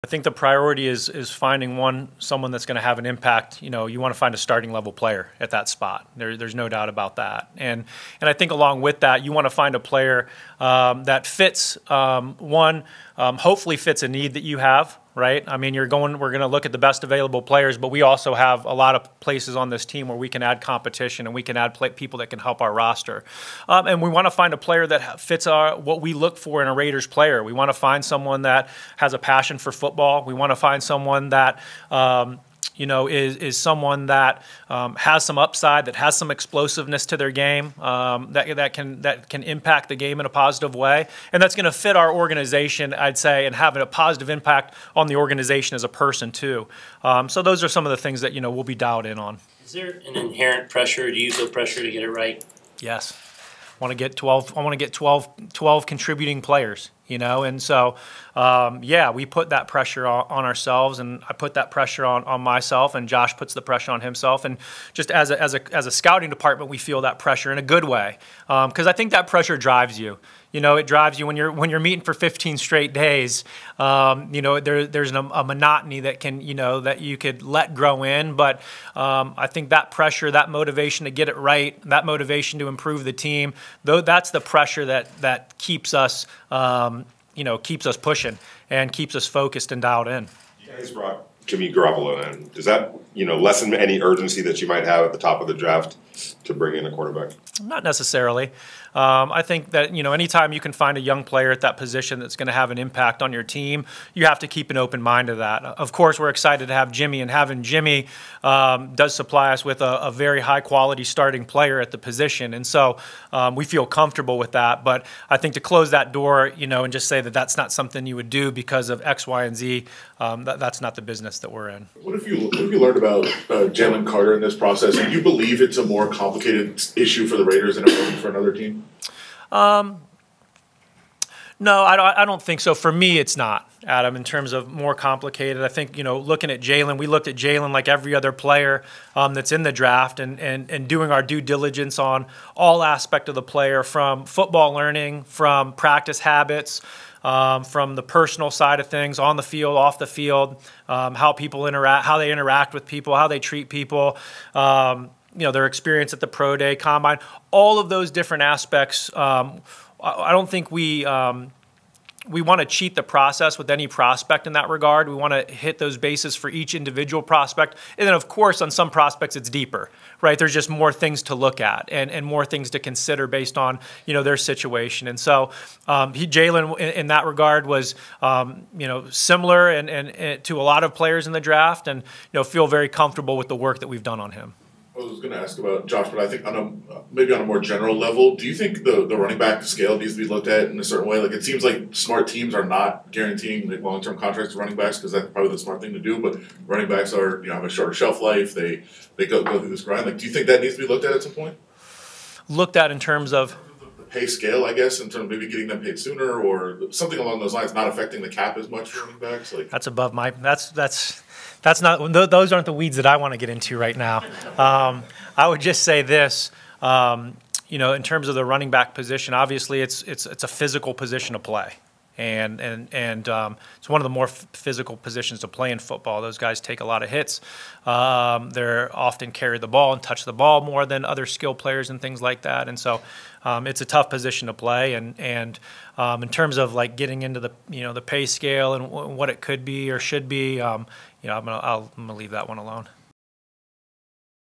Raiders GM Dave Ziegler talks pressure ahead of the 2023 NFL Draft
The Raiders have 12 picks leading into the 2023 NFL Draft and general manager Dave Zielger spoke about their vision for those picks, the pressure that exists for the brass to find playmakers, where they stand with their need at quarterback, what type of consideration they've given controversial defensive tackle Jalen Carter, and he updates on the latest with Josh Jacobs.